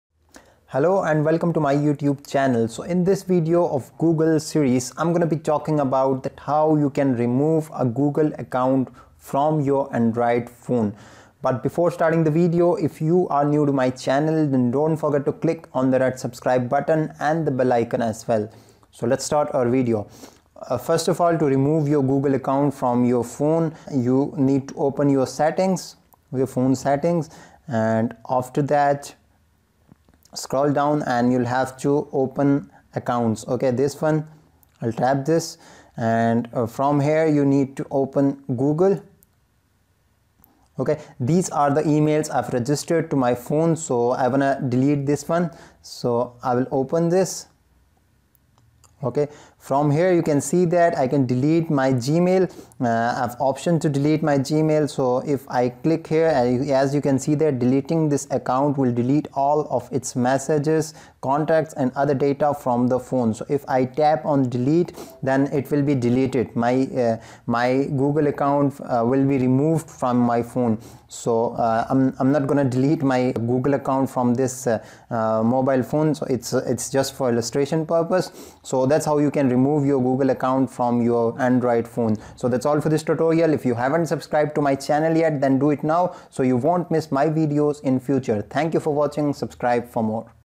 How to remove google account from Phone (Best Trick)
if this video was not helpful to remove your google account, then you can try 4uKey for Android

In this video, I have shared the trick to remove google account from your Android phone. If you want to remove your google account from android, then it's difficult for some users to explore settings to delete gmail account from phone, so I decided to make a video on that.
Be aware that if you are gonna remove your gmail account from android phone then all messages, photos, videos and all data of that gmail will also be removed from your phone.